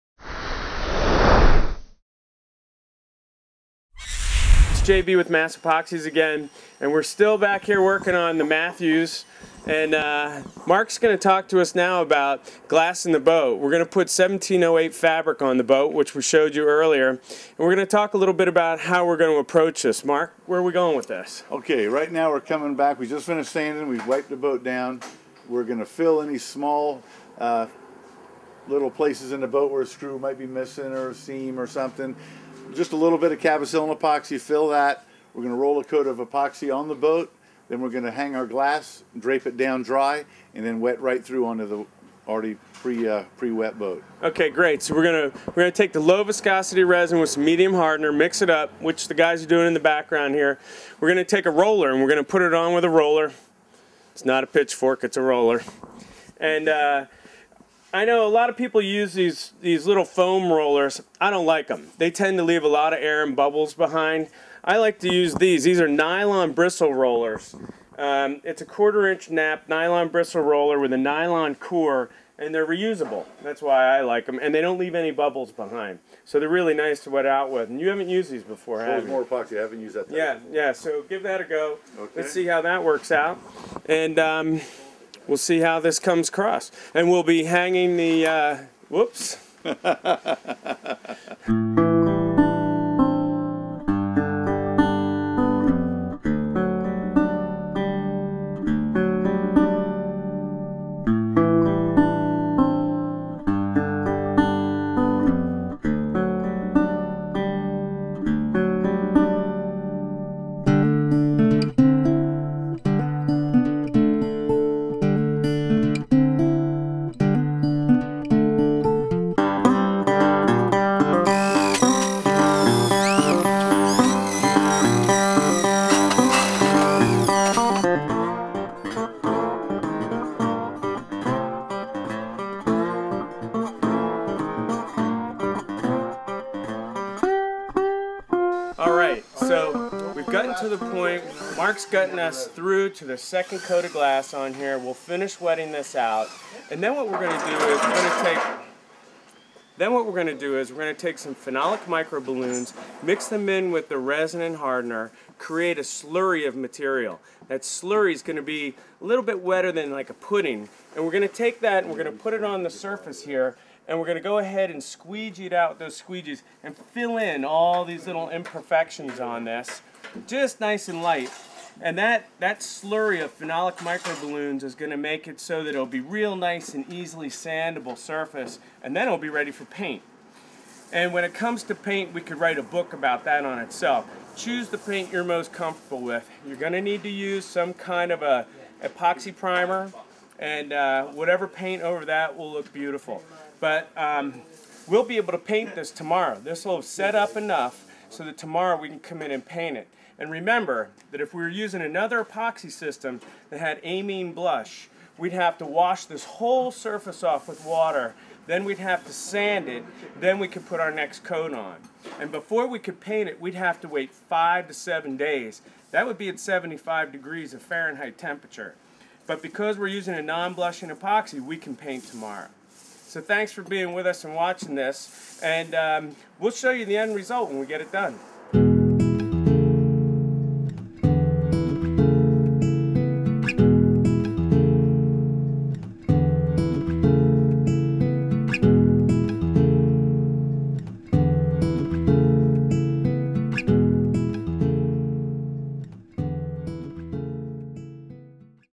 mas epoxies2
Restoring a  beautiful 1953 Matthews boat using mas epoxies. Products used: Cell-o-fill, phenolic micro balloons, colloidal silica, wood flour, milled fibers, 1708 biaxial cloth, vynil gloves, mixing sticks, plastic spreader, and mixing cups.